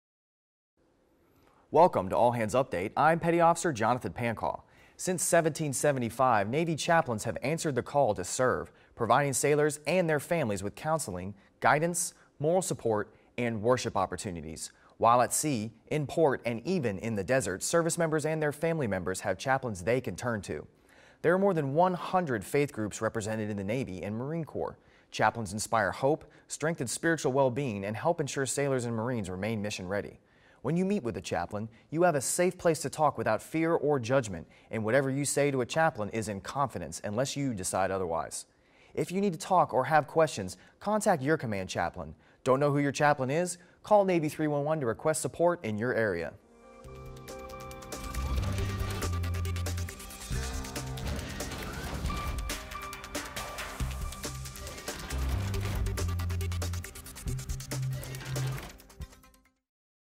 Chaplains Available to Assist Sailors, Families
Since 1775, Navy chaplains have answered the call to serve, providing sailors and their families with counseling, guidance, moral support and worship opportunities.